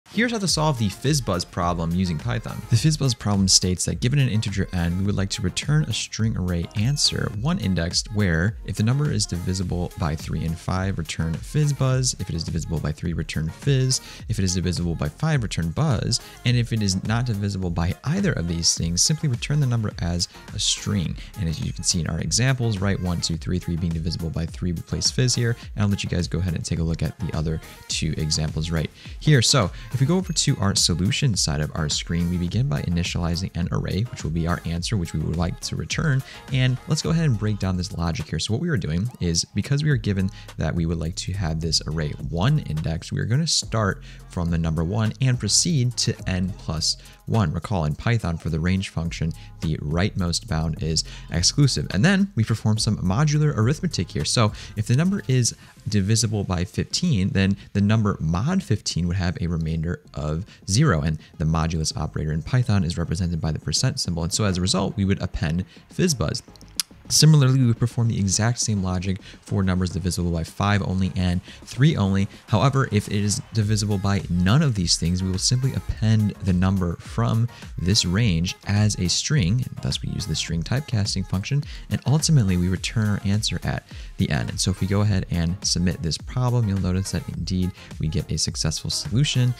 How to solve the FizzBuzz coding problem in Python
Learn the solution to the FizzBuzz coding problem using Python